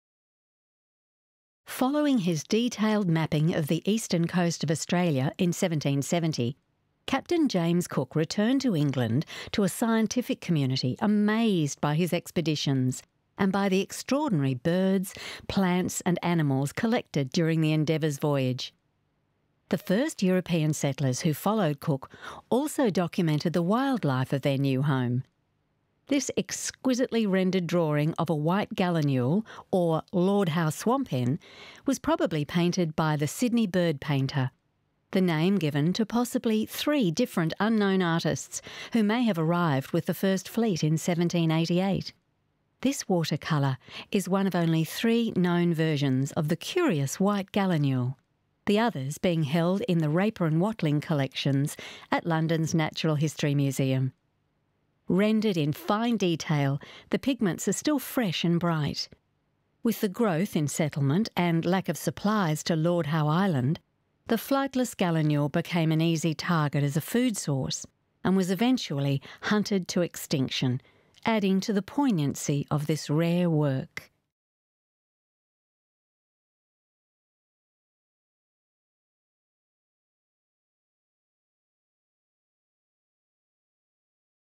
01 28287 - The Sydney Bird Painter - 'The white gallinule' c. 1791-92
The Sydney Bird Painter
The white gallinule c. 1791-92
Purchased 2000
National Gallery of Australia

Following his discovery of the eastern coast of Australia in 1770, Captain James Cook returned to England to a scientific community amazed by his expeditions and by the extraordinary birds, plants and animals collected during the Endeavour's voyage.

The first white settlers who followed Cook also documented the curious wildlife of their new home. This skilfully rendered drawing of a White Gallinule was probably painted by the Sydney Bird Painter, the name attributed to possibly three different unknown artists, who may have arrived with the First Fleet in 1788.

'A peculiar bird, the White Gallinule was one of a multitude of fauna first discovered on Lord Howe Island when Lieutenant Ball landed there in the Supply in March 1788. Like the unfortunate Mount Pitt bird of Norfolk Island that the Sydney Bird Painter also depicted, the White Gallinule was flightless and tragically hunted to extinction. The skin of only one White Gallinule has survived white settlement, enhancing the poignancy of this rare contemporary drawing.' (1)
Anne McDonald

(1) Richard Neville, Sotheby's catalogue, Sale AU645, Willcox Collection, Melbourne, 8 November 2000, Lot 4.

From: Anne Gray (ed), Australian art in the National Gallery of Australia, National Gallery of Australia, Canberra, 2002